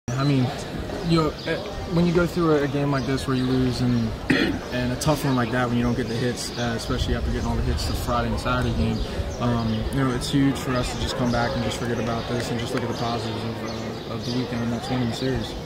Jake Fraley On LSU Moving Forward [4.13.14]
LSU freshman OF Jake Fraley discusses what the Tigers must do moving forward from their loss against Arkansas on Sunday.